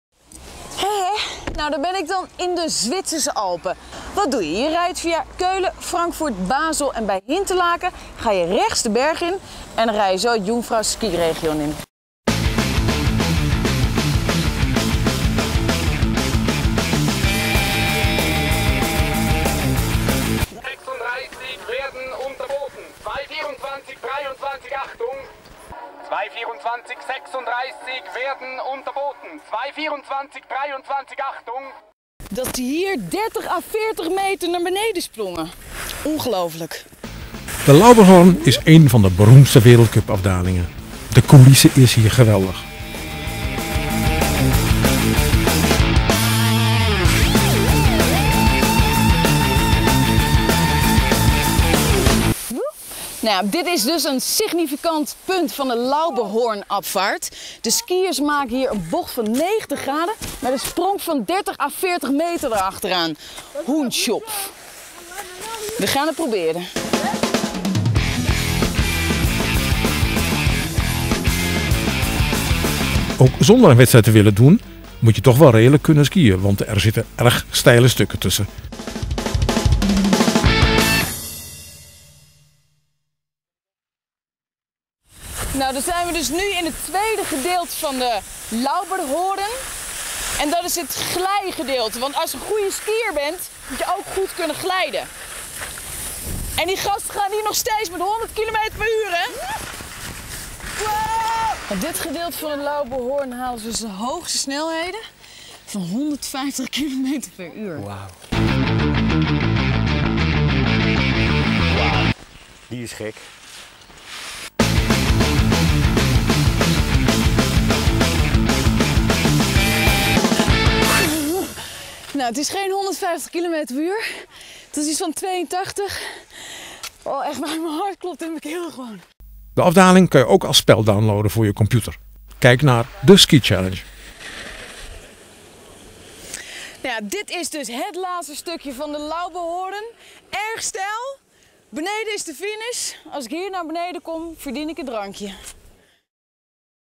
Wengen Lauberhorn Wereldcup afdaling
Wengen in Zwitserland is een autovrij skidorp. Er rijden alleen electrowagenetjes en als je naar het hotel wil vanuit het dal, dan moet je ook met de trein. Het skigebied is met dat van Grindelwald verbonden. De bekendste afdaling is de LAuberhorn, een wereldcup afdaling